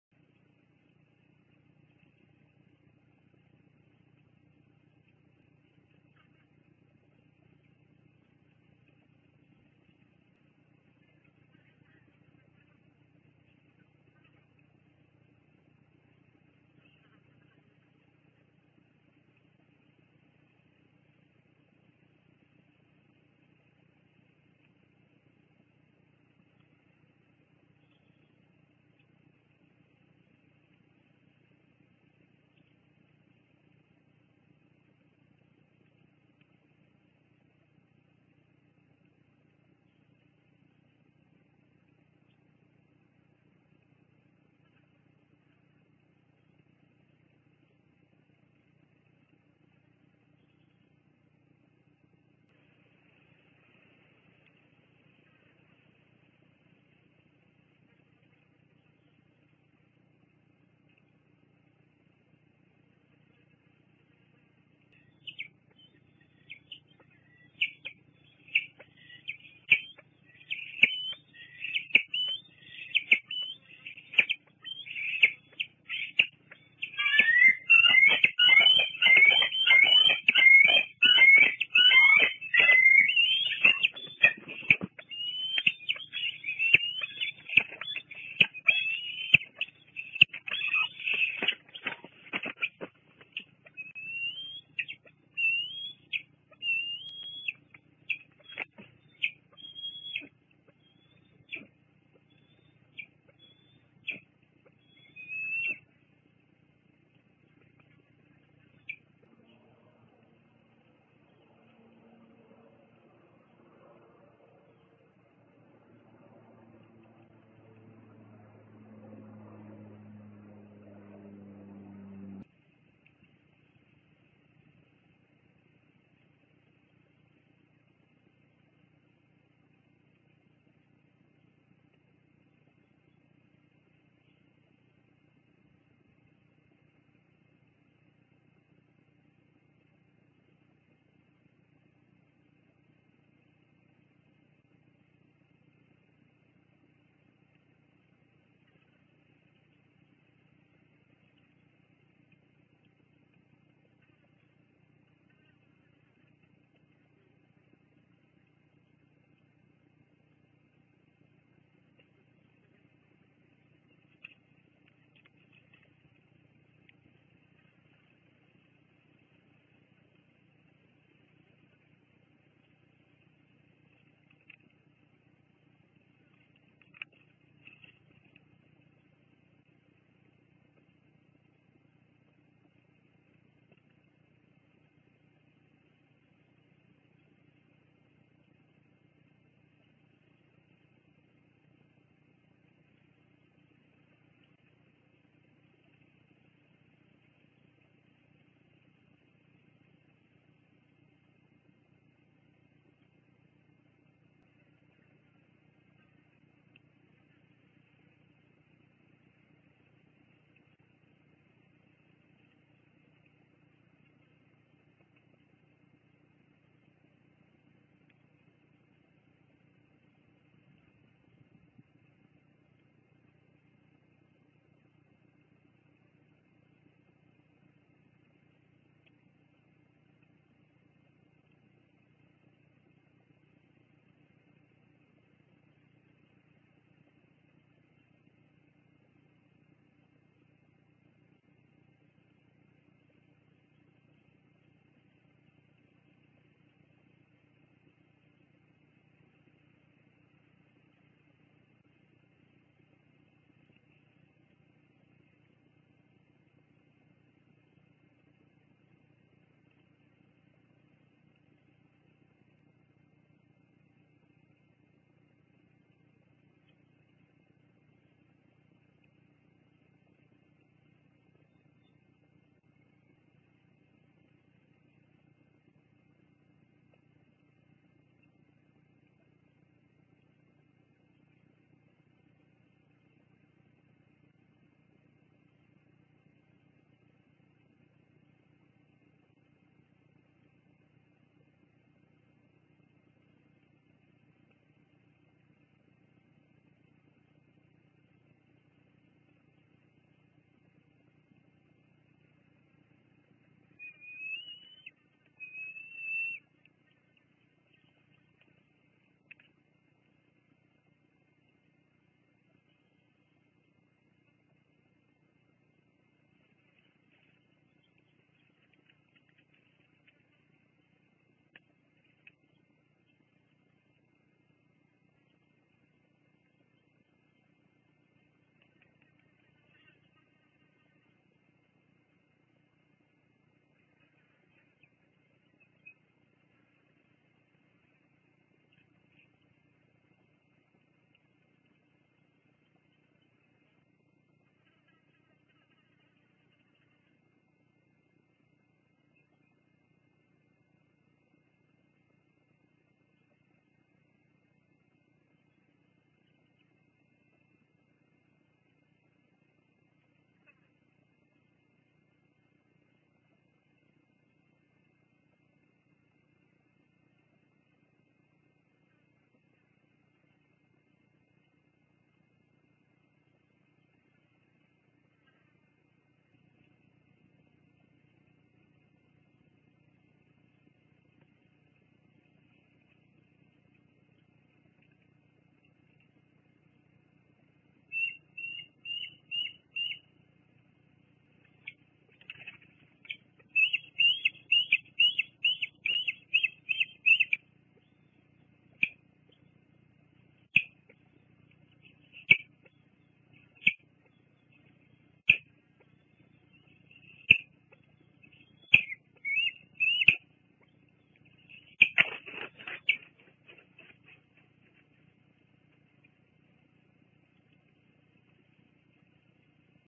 07-02-22 Fraser Point Bald eagles; Lilibet out and about, exploring the area, Explore.org.
Lilibet is found out and about exploring the area around the nest and feeling the ground underneath her feet.

Captured and edited by nighteagle007, through the live cam(s) provided by the Institute for Wildlife Studies, and Explore.org, 2022.  To view the live cams or to support these live cams and/or make donations,